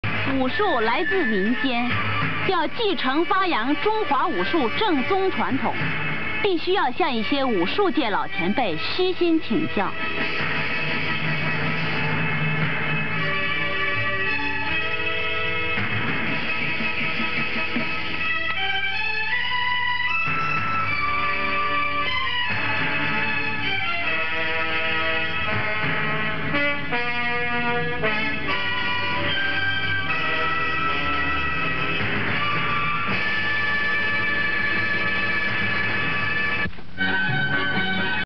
Jet Li and Traditional Masters
A very short clip of a young Jet Li discussing internal martial arts with some traditional masters. The teachers include Wu Tunan, Li Ziming, and Sun Jiayun.